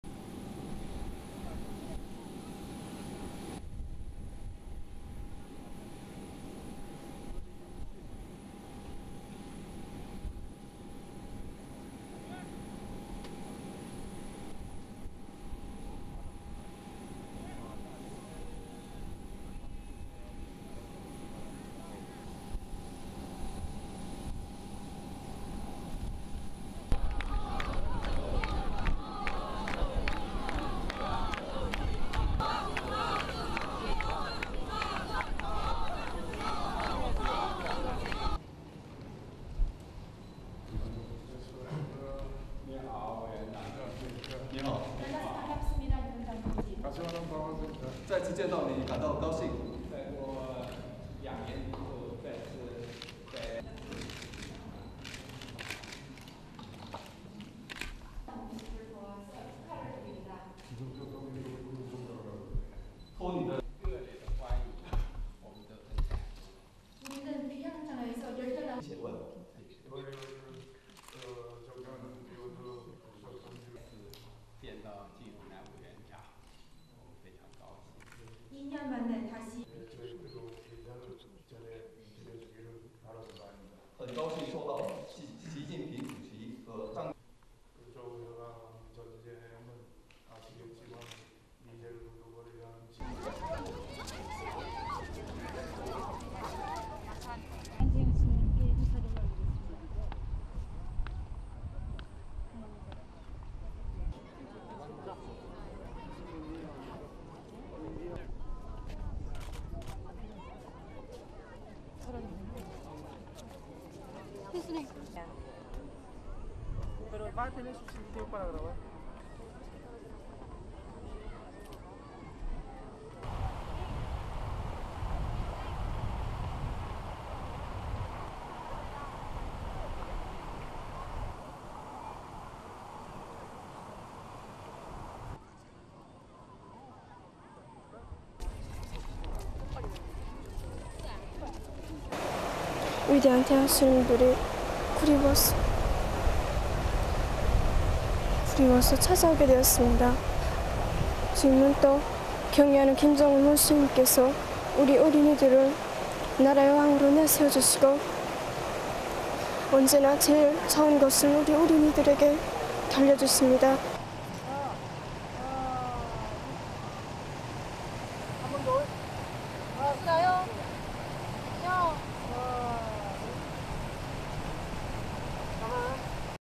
Chinese VP arrives to attend events to mark 60th anniversary of the end of Korean War
AP TELEVISION
1. Wide of Air China plane on tarmac at airport with red carpet leading to plane
2. Chinese Vice President Li Yuanchao walking down steps and shaking hands with Yang Hyong Sok, Vice Chairman of Standing Committee of Supreme People's Assembly
3. Li and Yang walking past honour guard
4. Various of Li and Yang walking past people waving flowers
5. Wide of Li entering room, shaking hands with Kim Yong Nam, Chairman of the Presidium of Supreme People's Assembly, zoom in to Kim and Li shaking hands
6. Li and Kim shaking hands
7. Cutaway of journalists
8. Tilt down from mural to Li and Kim speaking
9. Various of meeting
10. School children smiling while walking at Kumsusan Palace of the Sun
11. Children bowing in front of portraits of North Korean leaders Kim Il Sung and Kim Jong Il at Kumsusan Palace of the Sun
12. Wide of portraits of Kim Il Sung and Kim Jong Il
13. School children bowing then walking away
14. Various of memorial palace
15. Various of volunteers maintaining grass on memorial
16. Wide of North Korean flags
17. Wide of school children walking at memorial
18. SOUNDBITE: (Korean) Ro Yong Gyong, 12-year-old student from Pyongyang Changgwang Senior Middle School:
"I came here as I missed and yearned for the great generals (Kim Il Sung and Kim Jong Il). The respected marshal Kim Jong Un calls us, the children, the king of the country, and always gives the best to the children."
19. Tilt down from sky to school children having their class photo taken at memorial
20. Close-up of smiling school children
STORYLINE
Chinese Vice President Li Yuanchao arrived in Pyongyang on Thursday to attend events to celebrate the 60th anniversary of the end of the Korean War in which Chinese forces fought alongside North Korea.
He was taken straight to meet North Korea's head of state Kim Yong Nam at Mansudae Assembly Hall.
Li is the highest ranking Chinese official to visit North Korea since Kim Jong Un took over as leader upon his father's death in December 2011.
It is not clear yet if he has a meeting scheduled with Kim Jong Un, but both are expected to attend the military parade on Saturday which is the main commemorative event to mark the anniversary.
The trip lasting through to Sunday affirms a warming trend in relations between a deeply isolated Pyongyang and Beijing, its only real ally and key source of economic assistance.
The signing of the armistice that halted the 1950-53 Korean War is celebrated as a victory in North Korea.
The war ended on July 27, 1953 with North Korea signing an armistice agreement with the US-led coalition of forces under a United Nations Command.
The official view in North Korea is that the signing represents a victory by the isolated state over UN forces allied to South Korea, and the war is referred to as "the Fatherland Liberation War".
However the "final peaceful settlement" envisaged in the armistice never materialised, leaving the North still technically at war with the South.
The main event billed for the anniversary in North Korea will be a military parade through the centre of Pyongyang on Saturday.
In central Pyongyang on Thursday, groups of school children were being bussed into town to view the exterior of the Kumsusan Palace of the Sun, which is where the bodies of the country's founder, Kim Il Sung, and his son, Kim Jong Il lie in state.
Soldiers and volunteers were preparing the ground in front of the palace as preparations continued across the city ahead of Saturday's parade.